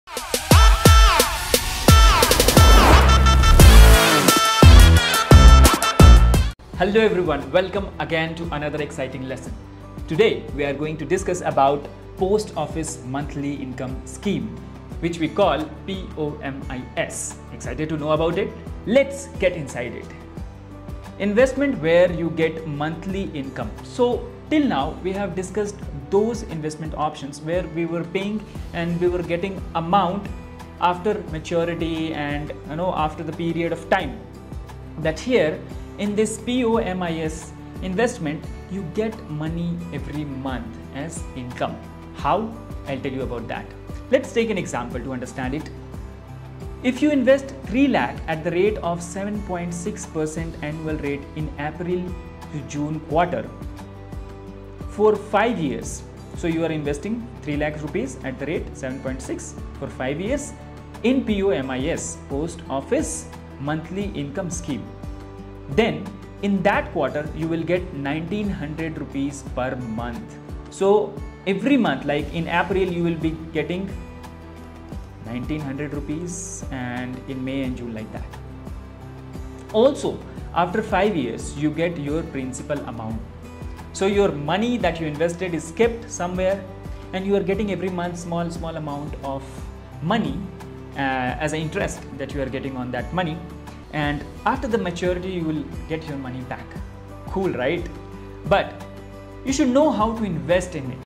What is Post office monthly income plan??
If you invest 3 lakh at the rate of 7.6% annual rate in April-June , for 5 years,  then in that quarter you get Rs 1900 per month interest, Also after 5 years you get your principal amount.